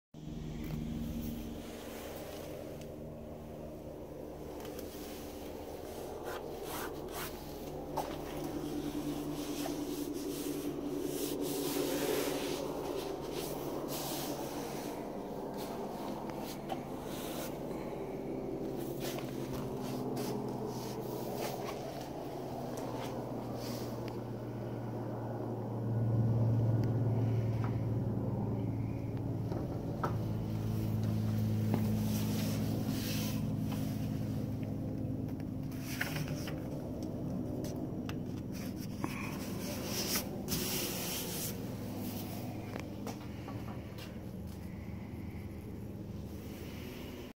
old canoe covered by flex seal spray (5 cans). Do not use Flex Seal for brush.
use spray only. do not use flex seal paint for a brush.